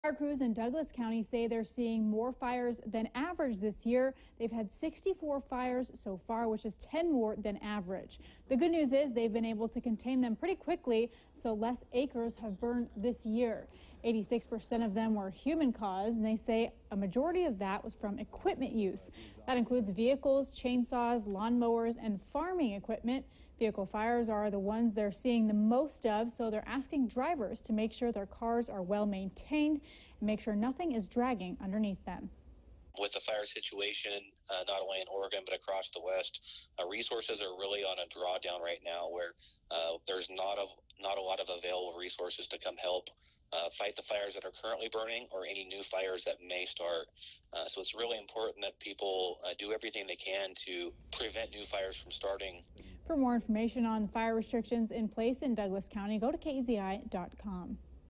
Crews in Douglas County seeing more fires than average
"It's really important people do everything they can to prevent new fires from starting," said Kyle Reed, Fire Prevention Specialist.